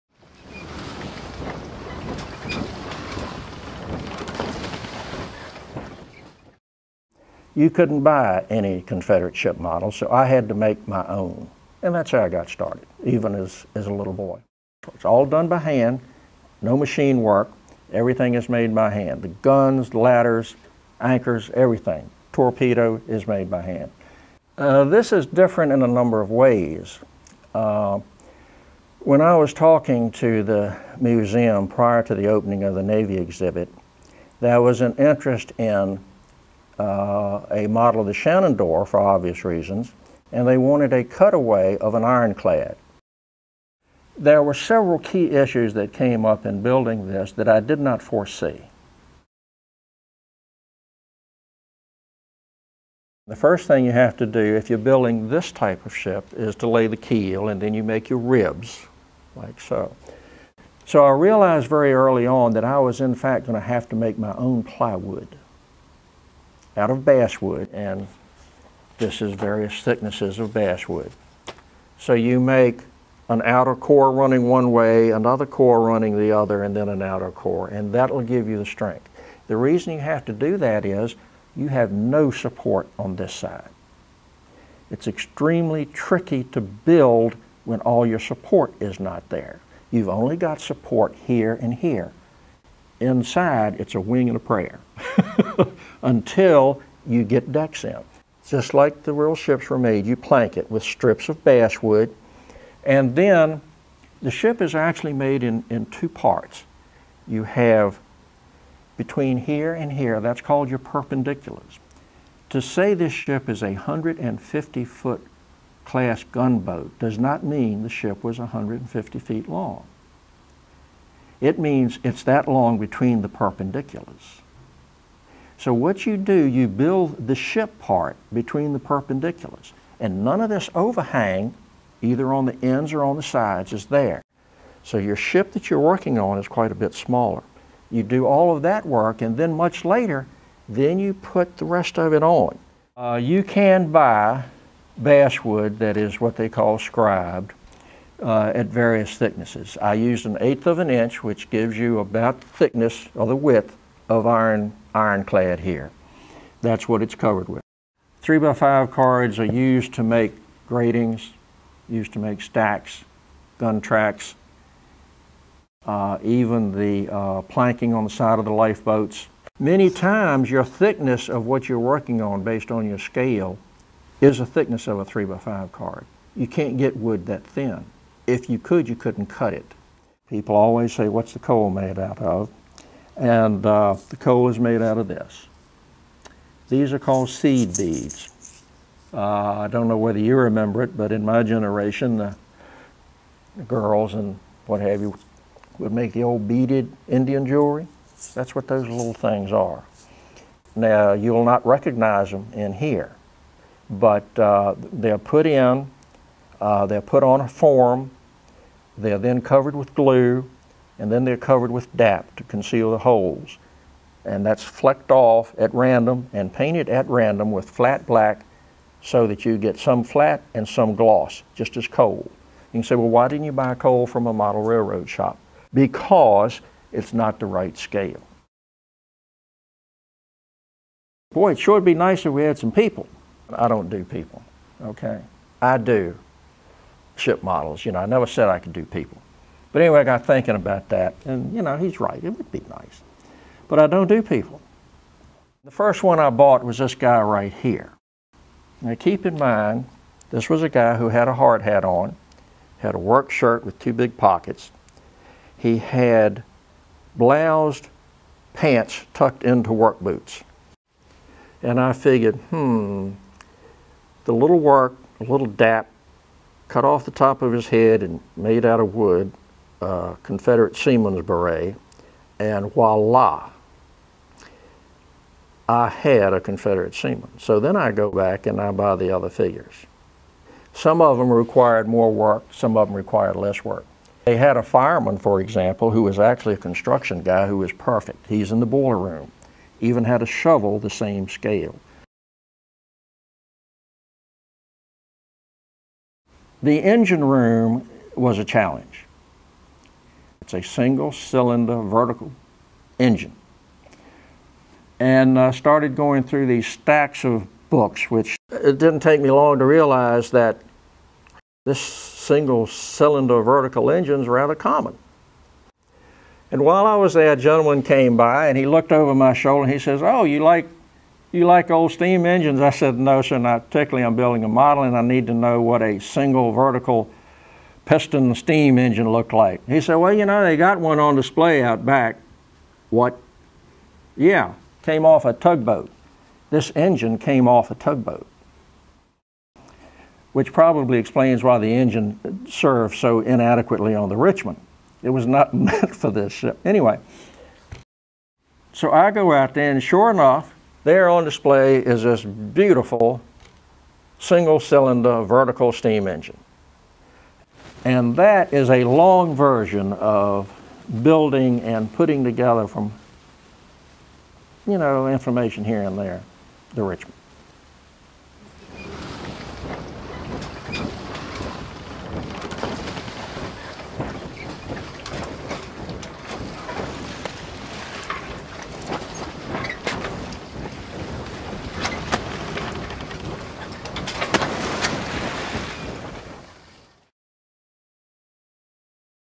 From Ironclad to Cardboard: the Model of the CSS Richmond Part Two
Model shipbuilder Ozzie Raines details the building of the model of the CSS Richmond. A unique "cutaway" model, the Richmond shows below deck views of the interior of the ship, the engine room, and features a moving vertical steam engine. Raines explains his use of specific materials and the troubleshooting process when building a model without plans.